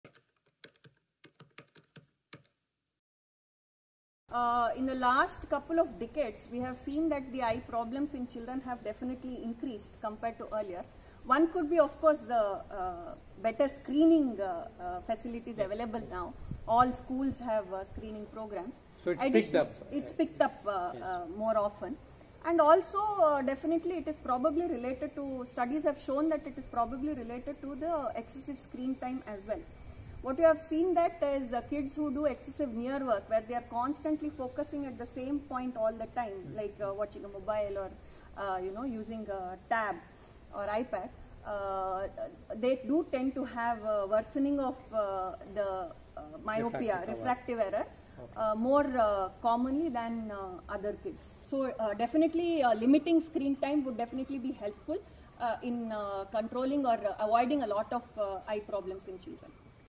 Does watching Mobile/ laptop affect Your Child's Eyes ?? Dr Swathi (Paed Opthal)
This video is a part of a detailed discussion with Dr Swathi Mailankody (Pediatric Ophthalmologist) about squint in children. In this segment the Doctor explains about the role of screens in damaging the vision of children.